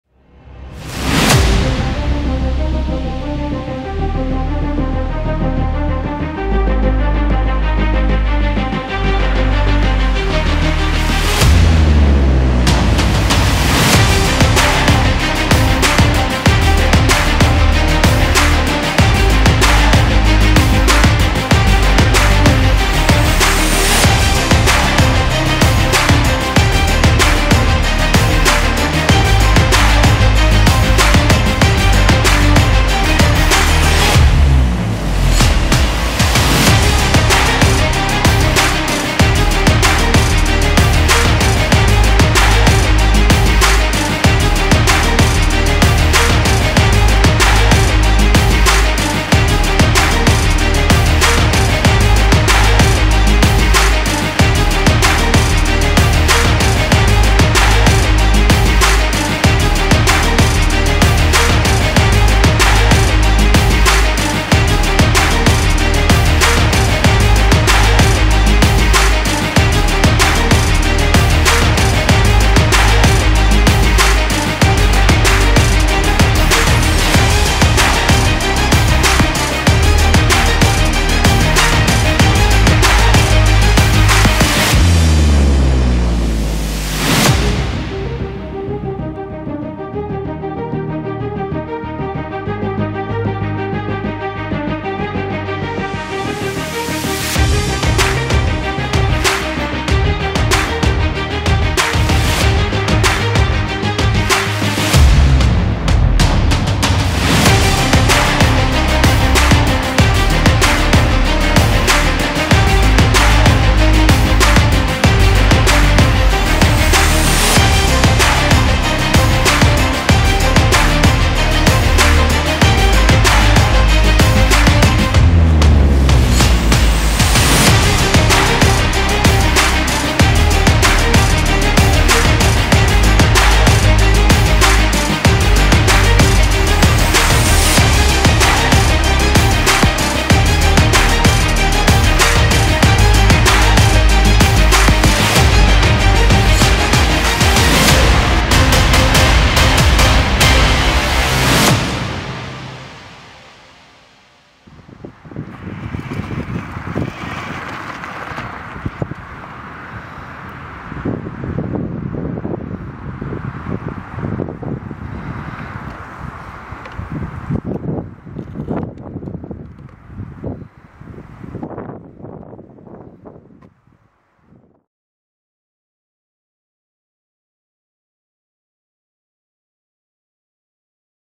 Buggy Man | Buggy Gokart 48V 1000W | Canon R6 I Sigma 24-70 f/2.8 | Dji Mini 2
Buggy Gokart 48V 1000W filmed with Canon R6 paired with Sigma 24-70 f/2.8. Drone scenes done with Dji Mini 2.